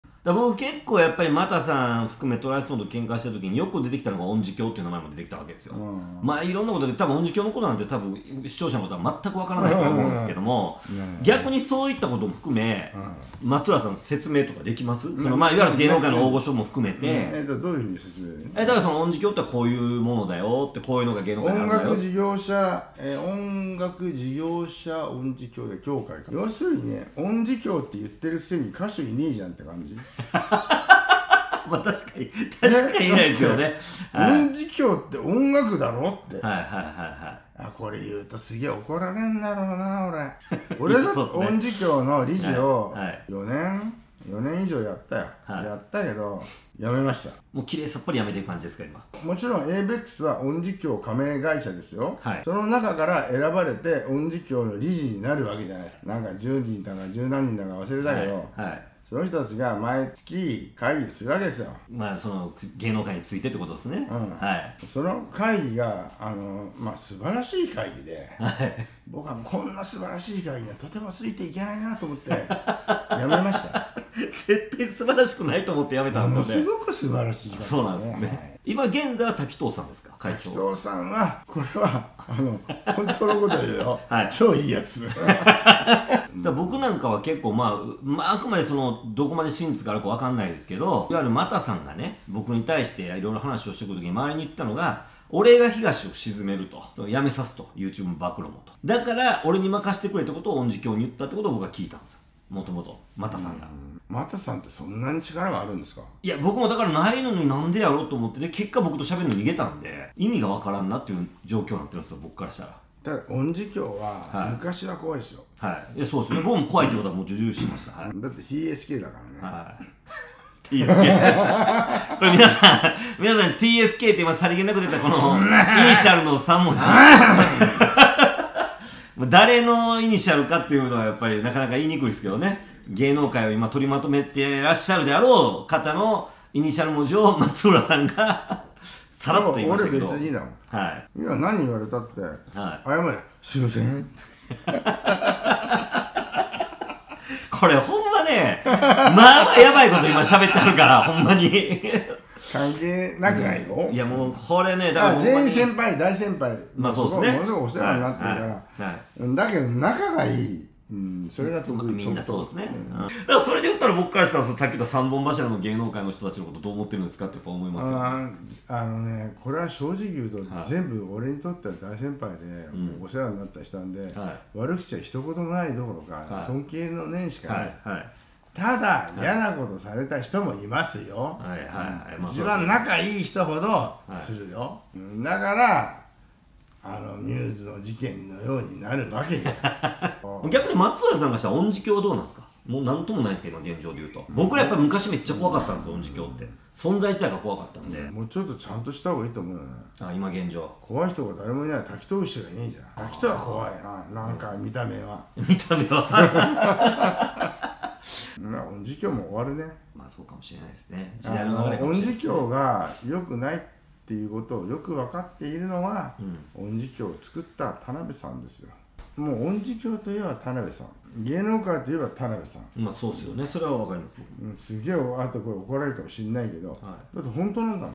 【松浦勝人×ガーシー】ガーシーを狙っている組織の闇を暴きます！この組織はこのままだとマジで終わるよ!!【切り抜き/avex会長/東谷義和/暴露/芸能界/ガーシーch】
切り抜いてる配信のフル映像は下記のメンバーシップ内からご覧になれます！

【松浦勝人×ガーシー】ガーシーが恐怖を抱く音事協という組織について語る!!この組織はこのままだとマジで終わるよ!!【切り抜き/avex会長/東谷義和/暴露/芸能界/ガーシーch】